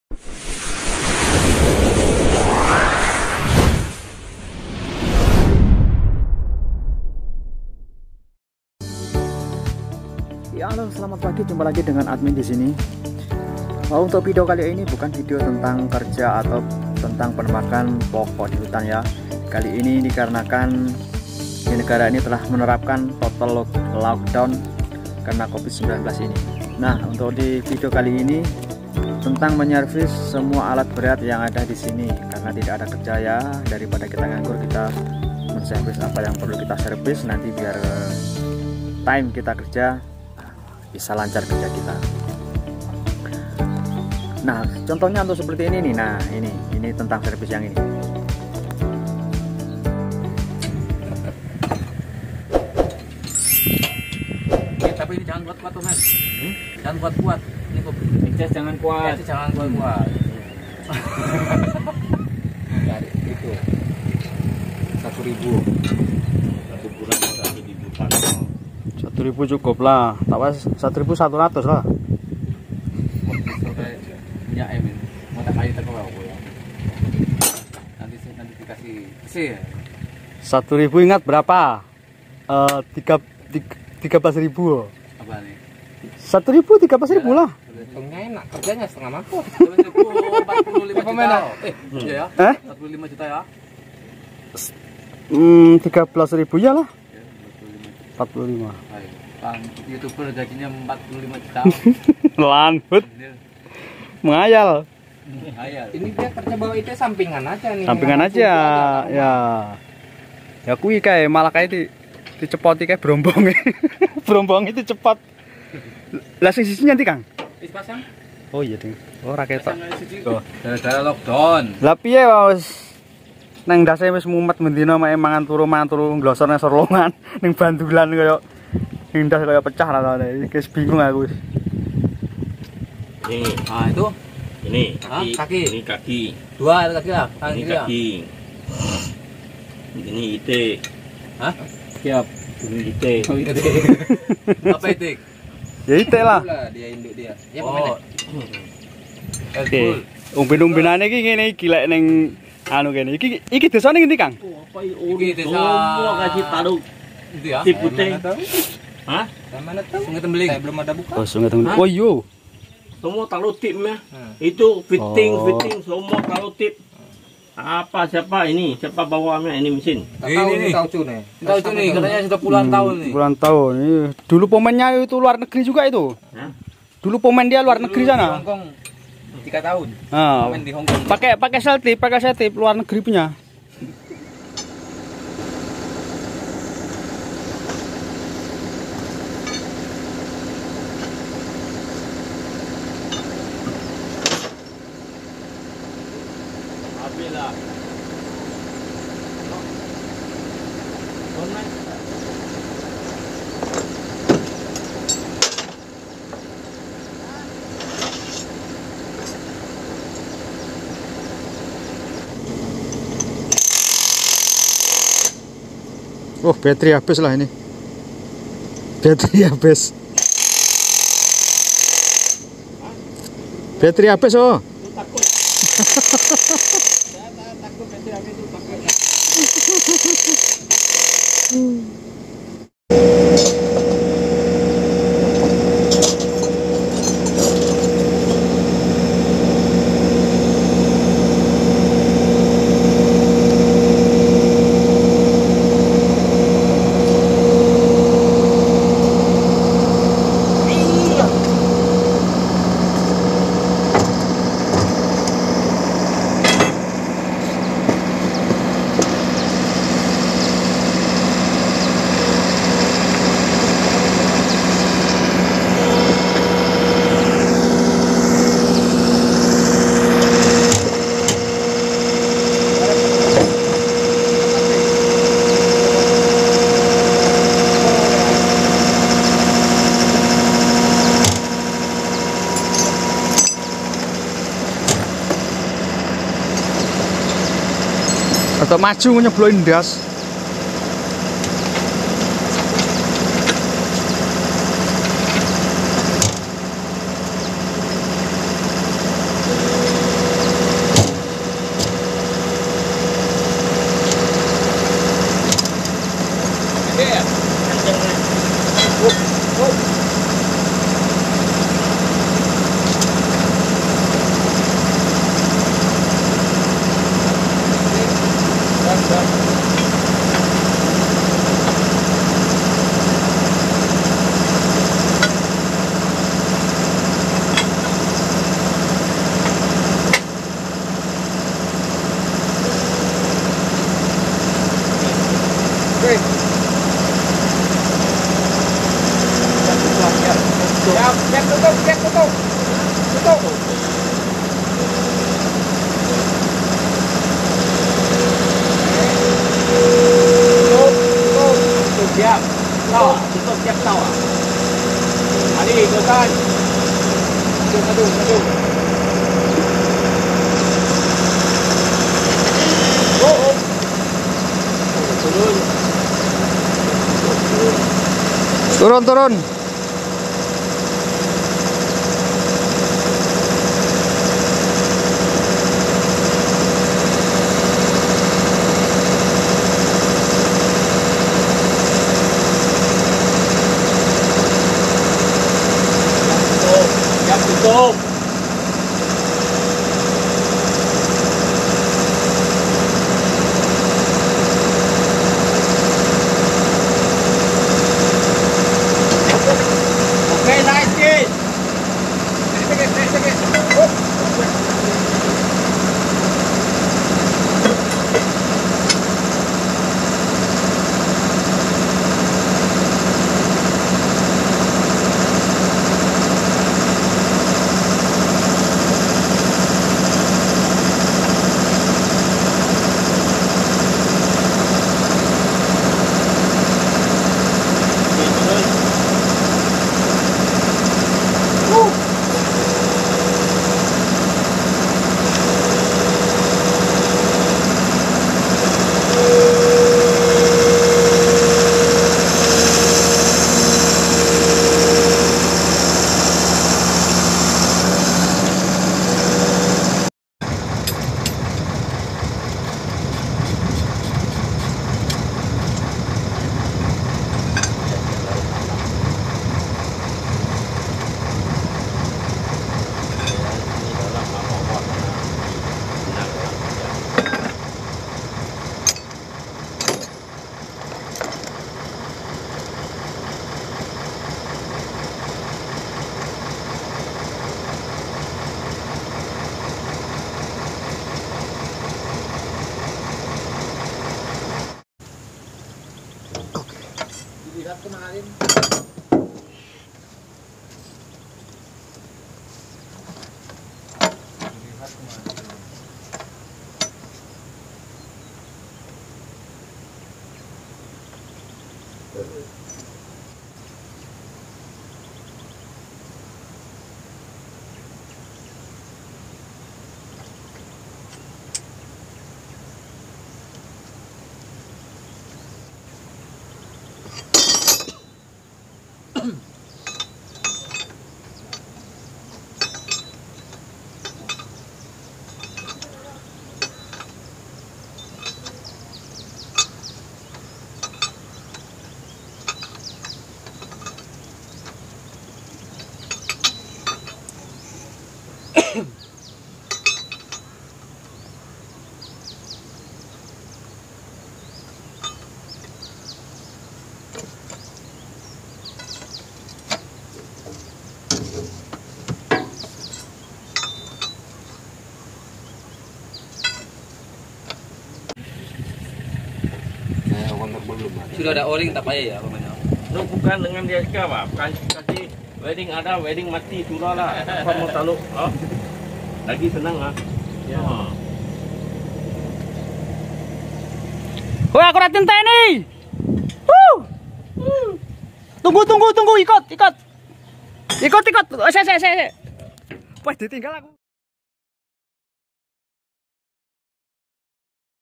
video rekaman kerja balak dalam hutan Malaysia terdampk lockdown covid19
video rekaman kerja balak dalam hutan malaysia terdampak lockdown covid-19 yang diterapkan di negri Malaysia ini,inilah cara memper baiki kontrol bal excavator
Garbage disposal system
วิธีการให้บริการการควบคุมก้อนของรถขุด
làm thế nào để bảo dưỡng điều khiển thùng máy đào